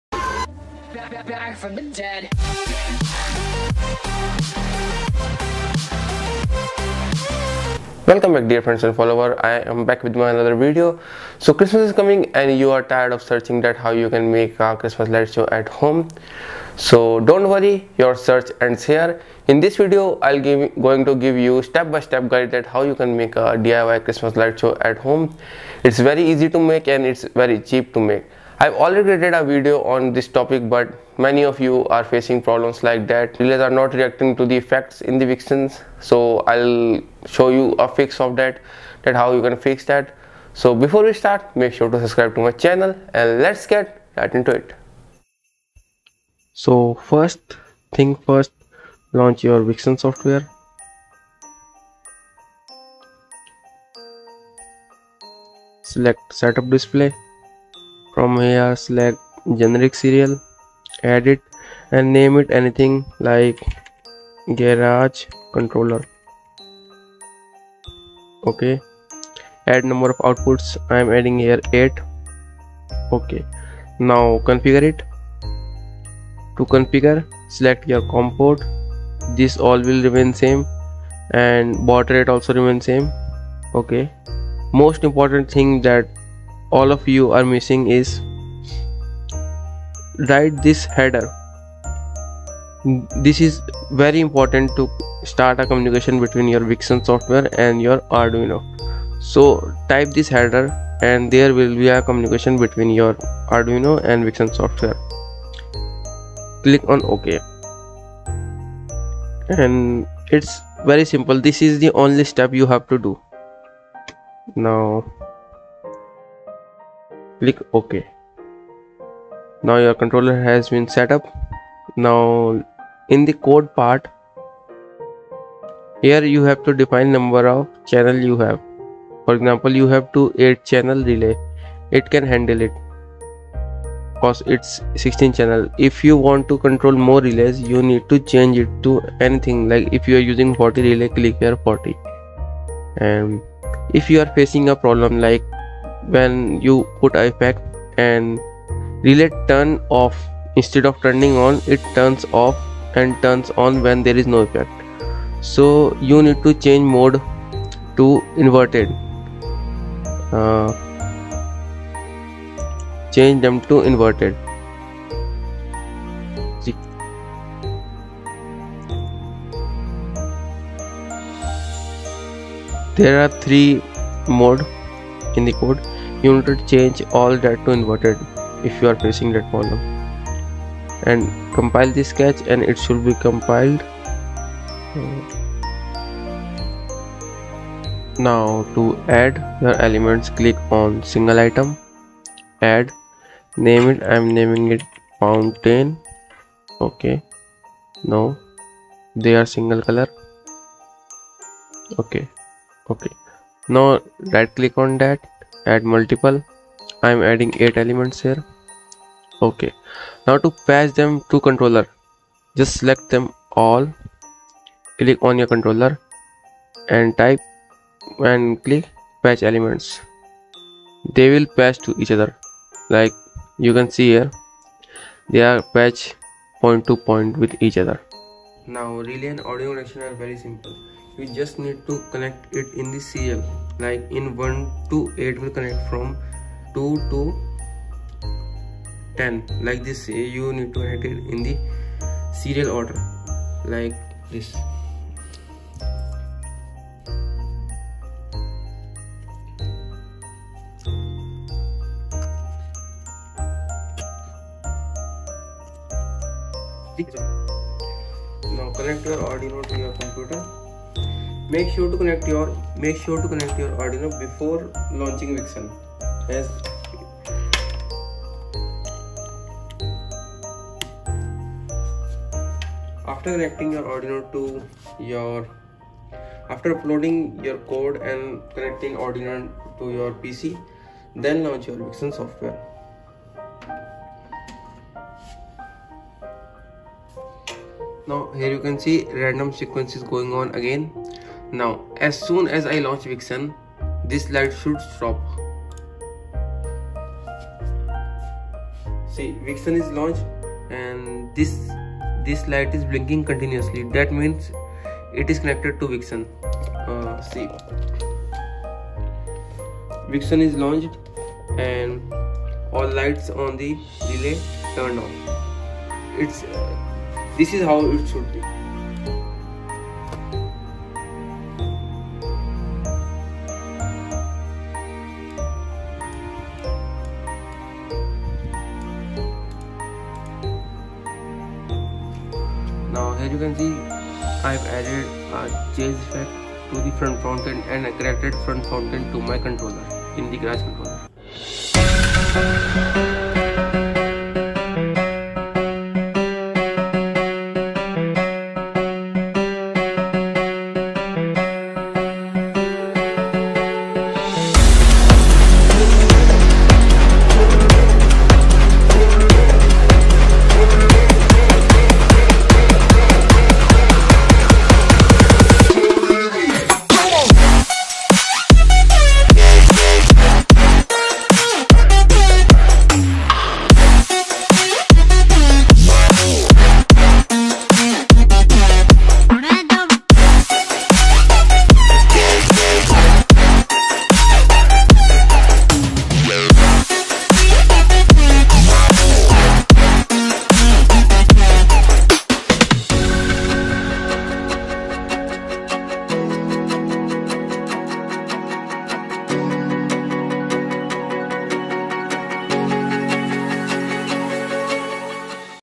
How to make Christmas light show with vixen using arduino and relay (DIY).
Hello in (How to make Christmas light show with vixen using arduino and relay.) video i will show you how you can make christmas light show using arduino and relay and program it with vixen 3. Here i had not used light o rama because for that you will need light o rama control box. and that is too costly. In this video you can make light in a very cheap manner. here basically i will show you how to connect arduino and relay to vixen.
THINGS YOU WILL NEED:
1. ARDUINO MEGA
2. RELAY
3. JUMPER WIRE
4. BREADBOARD